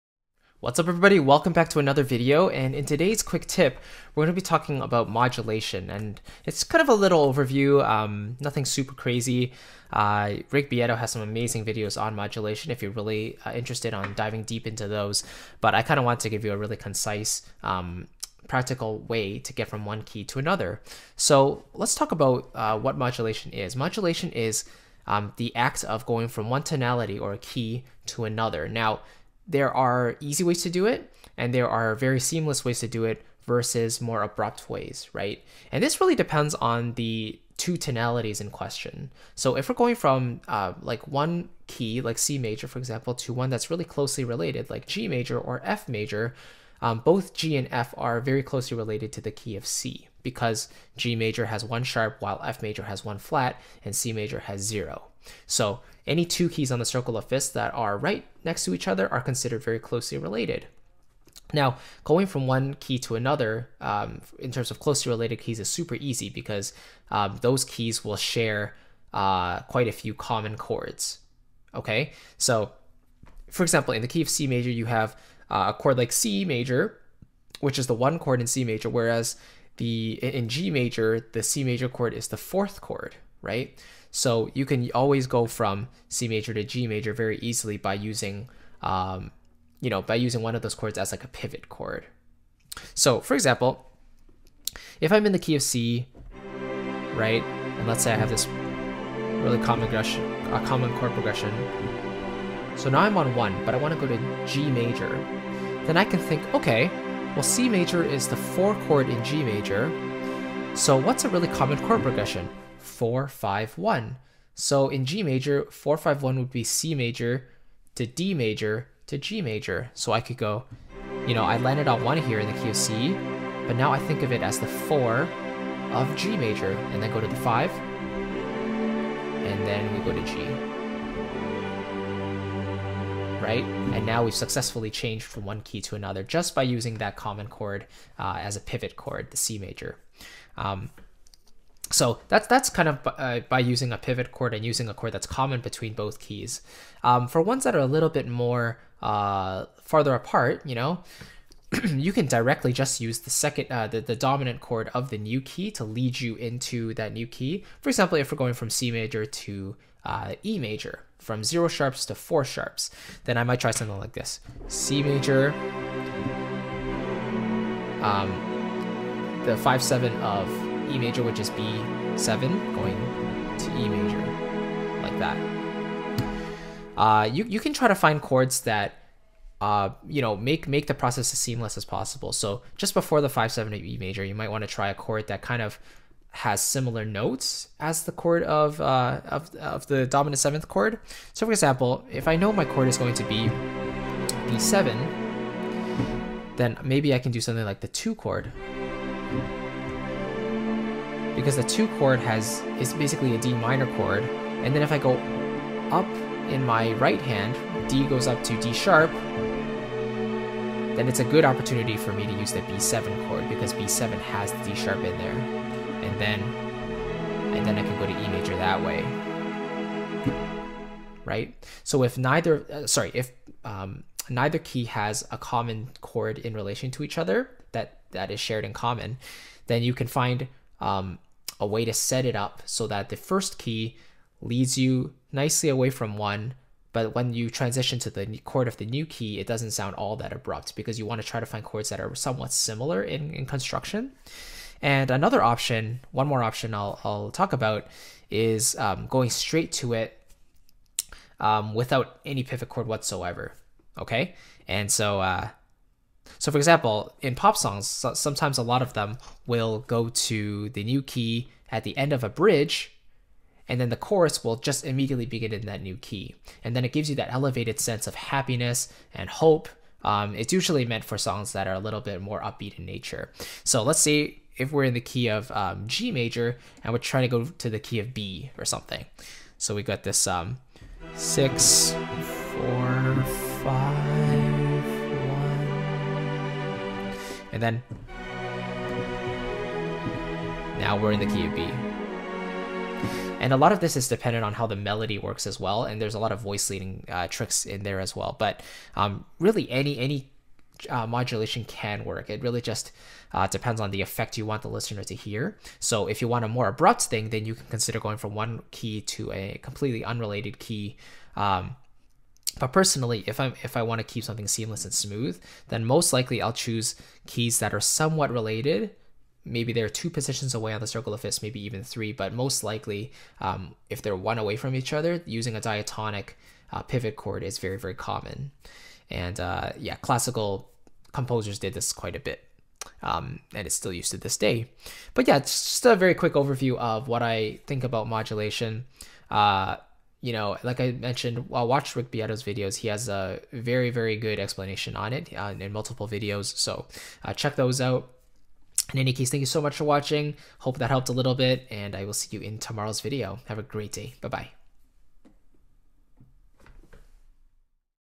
How To Modulate: 2 Simple Ways!
►► Learn the fundamentals of music theory that every composer needs to know.

How do you go from 1 key to another? There are many many ways, but let's discuss 2 methods in this video. Enjoy!

Evenant has some wonderful courses on music production, composition, and design. Check them out here!